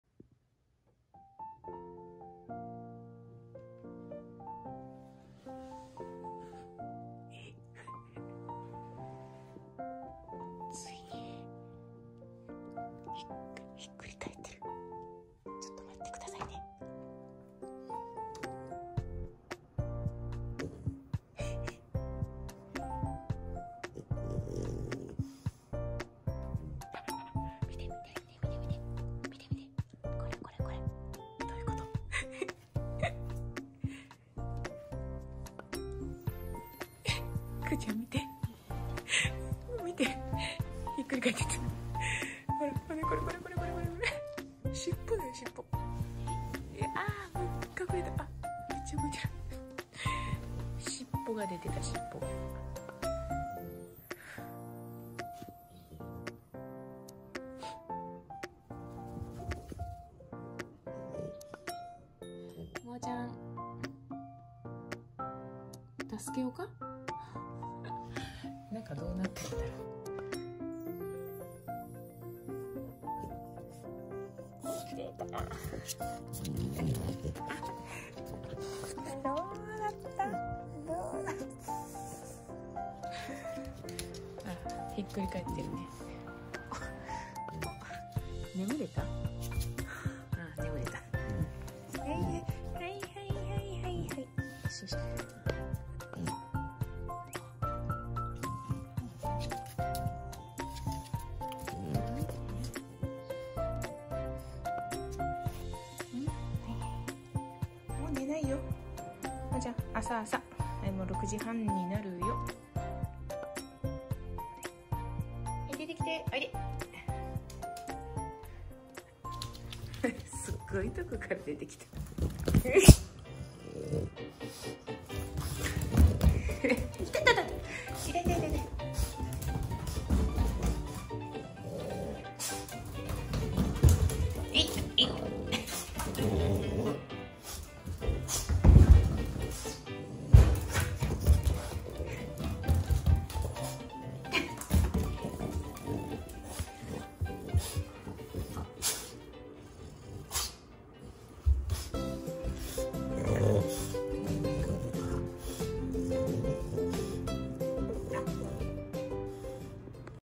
【元保護犬フレブル】ついに！…天地がひっくり返った朝…
皆さんこんにちは☺️いつもご視聴頂き、誠に有難うございます☺️フレブル★モアナチャンネルです🤗

4月17日（土）…今朝６時過ぎ…いつもの様に…モアナの鼻息で目覚め😂…よいこらしょと、寝起き、一番痛みのある腰をあげましたら😂💦…なんということでしょう😆💦…ついに…ついに！！…モアナ🐽かまくらベッドごと、ひっくり返っておりまして🤣💦…

以前、動画にさせて頂きましたが…飼い主達が寝静まり…草木もねむる丑三つ時😂…モアナ🐽✨密かに何かをしているようで😂💦…朝目覚めると、かまくらベッドがあっち向いてたり…こっち向いてたり…挙げ句の果てには、えんとつの様に、上を向いていたりと😂💦…まぁとにかく、何をやっているんだか…いつからか、朝、そんな風になっていました😅💦…

最近も、相変わらず、かまくらベッドがおかしな位置に移動していたりするのですが…今朝はついに…かまくらベッドと共に…天地がひっくり返ったモアナでした🐽✨

いつも、いいねやコメントを頂き、誠に有難うございます☺️また、チャンネル登録におきましても、心より感謝の気持ちでいっぱいです☺️⤴︎⤴︎…今後とも、モアナ🐽と飼い主…アラフィフコンビを、どうぞ宜しくお願い致します🤗

【フレブル★モアナチャンネル】

お借りした素敵な楽曲♬